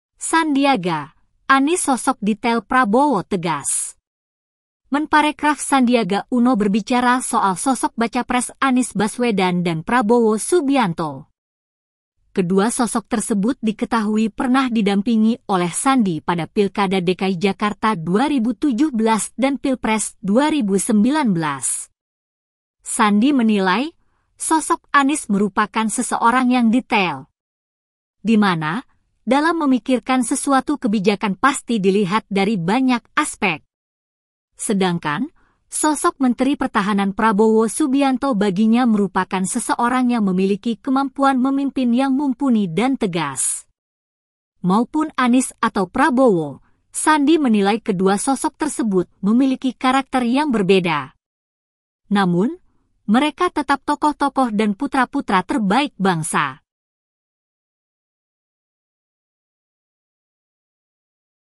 Sandiaga Uno: Anies Baswedan Sosok Detail, Prabowo Subianto Tegas
TRIBUNSOLO.COM - Menteri Pariwisata dan Ekonomi Kreatif (Menparekraf) Sandiaga Uno berbicara mengenai sosok bakal calon presiden (bacapres) Anies Baswedan dan Prabowo Subianto.

Kedua sosok tersebut diketahui pernah didampingi oleh Sandi pada Pemilihan Kepala Daerah (Pilkada) DKI Jakarta 2017 dan Pemilihan Presiden (Pilpres) 2019.

Sandi menilai, sosok Anies merupakan seseorang yang detail.

Di mana, dalam memikirkan sesuatu kebijakan pasti dilihat dari banyak aspek.

"Pak Anies ini adalah seorang yang sangat detail, dia memikirkan suatu kebijakan itu dari banyak sekali aspek," ungkap Sandi, dikutip dari YouTube Kompas TV, Senin (7/8/2023).

"Dan sebagai seorang pendidik ya karena latar belakangnya adalah pendidikan, beliau (Anies Baswedan) melihat banyak sekali dari sisi pendidikan," sambungnya.

Sedangkan, sosok Menteri Pertahanan Prabowo Subianto baginya merupakan seseorang yang memiliki kemampuan memimpin yang mumpuni dan tegas.

"Kalau Pak Prabowo ini kan tentara, seorang jenderal yang jenderalnya lapangan lagi."

"Jadi, beliau (Prabowo Subianto) memiliki kemampuan memimpin secara mumpuni dan tegas," ujar Sandi.

Maupun Anies atau Prabowo, Sandi menilai kedua sosok tersebut memiliki karakter yang berbeda.

Namun, mereka tetap tokoh-tokoh dan putra-putra terbaik bangsa.

"Jadi, kedua karakter ini super berbeda Pak Anies dan Pak Prabowo, tapi mereka adalah tokoh-tokoh dan putra-putra terbaik bangsa kita," pungkasnya.

Artikel ini telah tayang di Tribunnews.com dengan judul Begini Sosok Anies Baswedan dan Prabowo Subianto di Mata Sandiaga Uno,
Penulis: Rifqah